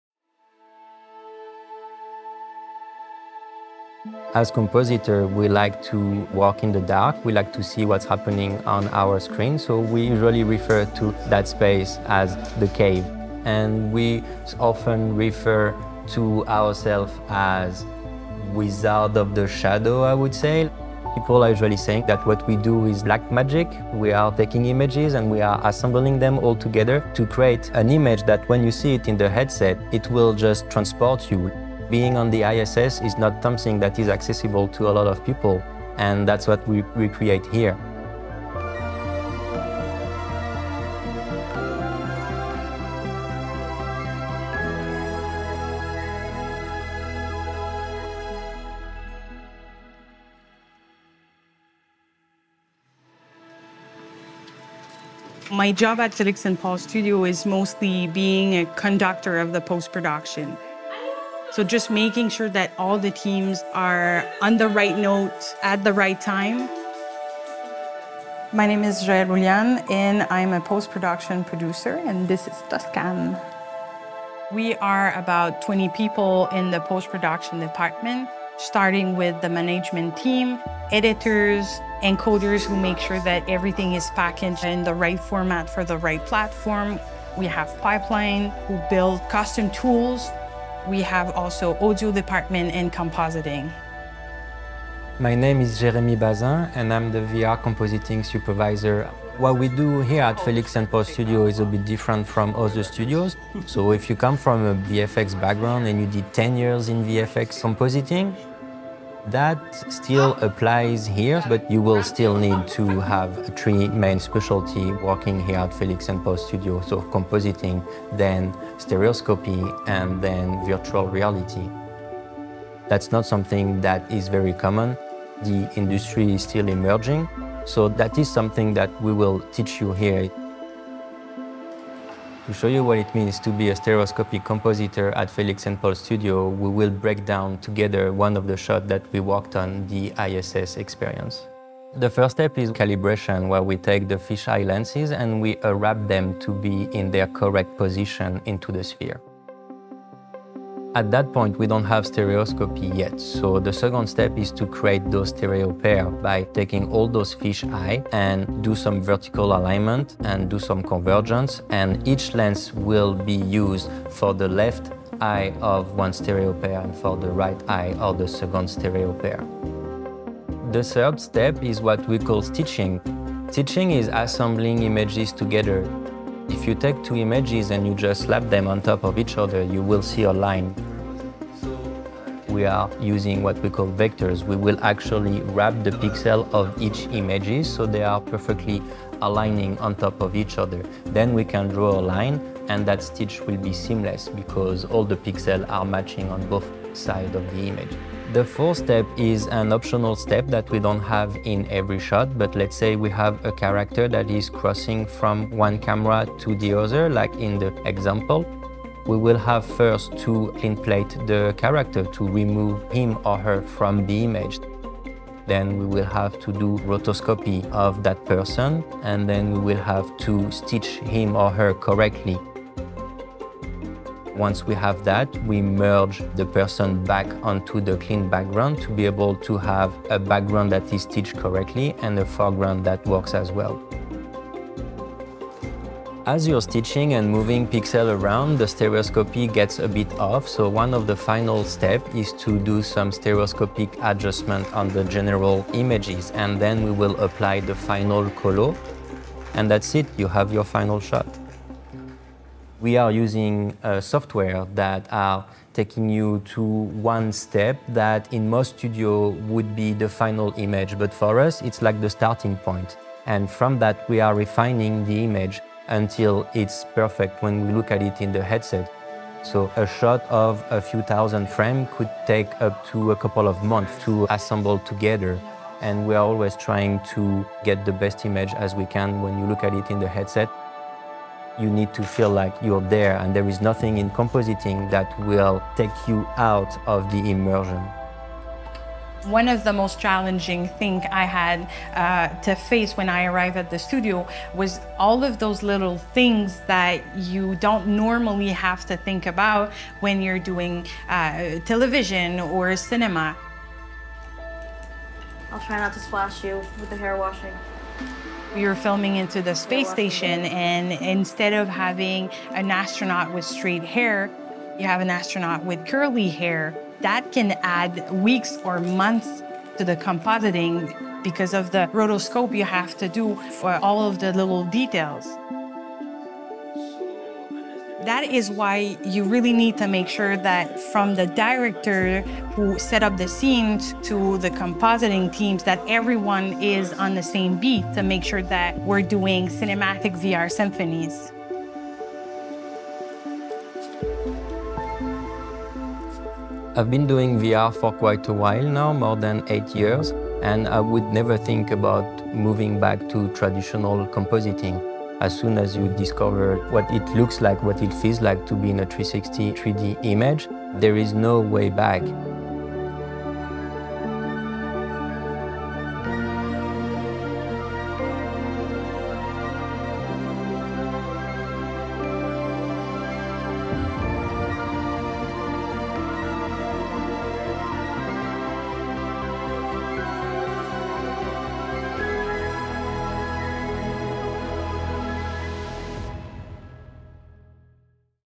Making Magic with VR Post-Production : Inside Felix & Paul Studios Episode 05
Go behind the scenes at Felix & Paul Studios where the post-production team turns raw 360-degree footage into polished and spellbinding stereoscopic VR. This episode offers insights into the detailed post-production process that meticulously shapes each frame, pixel by pixel. Observe the complex art of compositing and watch as a routine moment aboard the International Space Station gains depth and realism, evolving into a captivating visual narrative that takes months to achieve.
Explore the critical role of post-production management, where precision and timing are paramount, and gain understanding of the painstaking attention required to bring even the smallest details to life. Discover the behind-the-scenes intricacies that make each shot a masterpiece of visual effects, highlighting the unique challenges VR post-production faces compared to traditional cinema.

CHAPTERS
00:00 Wizards of the shadow
00:55 Post-production producing
01:35 Intro to stereo VR compositing
02:12 Compositing demo
04:13 The Felix & Paul Studios difference
04:48 Unique challenges of VR post-production

INSIDE FELIX & PAUL STUDIOS

Episode 5
Written by : Kalina Bertin & Ashley Duong
Directed by : Ashley Duong

Filmed with the Canon RF 5.2mm F2.8 L Dual Fisheye lens and Canon EOS R5 C camera

Creative Producers: Félix Lajeunesse & Paul Raphaël
Producer: Katarina Soukup
Executive Producer: Stéphane Rituit

Directors of Photography: Martin Gros & Daniele Tomelleri
Editor: Ashley Duong
Original Music: Jean-Olivier Bégin

PRODUCTION
Production Coordinator: Arnaud Héry
Assistant Camera: Daniele Tomelleri
Location Sound: Mike Ritchie

POST-PRODUCTION
Head of Post-Production: Jacques Levesque
Post-Production Producer: Joëlle Boulianne
Post-Production Manager: Arnaud Héry
Assistant Editors: François Beaupré-Goulet & Charles Tranquille
Sound Design, Dialogue Editing and Mix: Mike Ritchie
Technical Manager: Guillaume Malichier
Compositing Supervisor: Jérémy Bazin
Digital Imaging Technicians: Sylvain Thibodeau & George Rafie
Graphic Design: Nicholas Boissy
Motion Graphics: Faisal Lutchmedial
Colorist: Martin Gros
Archives: Felix & Paul Studios

MARKETING & COMMUNICATIONS
VP Brand & Product Marketing: Sébastien Roy
Director of Communications: Nedjma Belbahri
Marketing Coordinator: Marcia Griffith

WITH THE PARTICIPATION OF CANON
Kevin P. McCarthy
Melisa Hawjung Yoo
Brandon Chin
Takashi Akahane
Linda Milano
Eloise Marie Pisano
Antonino Gabriele Berretta
Aimee Isabelle Lindaas
Stephanie Caro

Infinity Experiences
Julie Tremblay
Valérie Gareau
Jose Garcia
Espace pour la vie
Montreal Planetarium
Olivier Hernandez
Josée Bolduc
Centre sportif du Parc olympique
Mathieu Dumont
Canadian Space Agency
Frédérick Fink
Maxime Vézina Laprise
PHI Centre
Caroline Clément